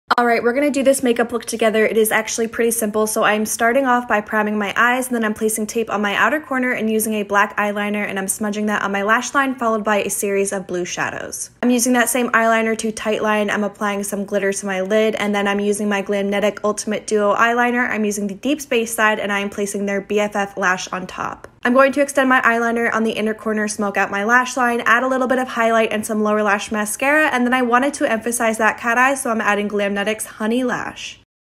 BLUE SMOKEY CAT EYELINER
I have been loving cat eye looks lately and I wanted to create a blue smokey cat eye so I could use my Lemonhead LA Houdini Spacepaste. I love the Houdini Spacepaste because not only is it gorgeous but I find the color varies slightly depending on what you have around it!

Products Used:
P.Louise Eyeshadow Base
@PersonaCosmetics 24 HR Waterproof Eyeliner in Black
@bperfectcosmetics669 x Stacey Marie Mua Love Tahiti Palette
@lemonheadLA Houdini Spacepaste
@item beauty Lash Snack Mascara
@Glamnetic Ultimate Duo Liner and BFF Lash and Honey Lash (code "BNB15" saves you 15% on their site!)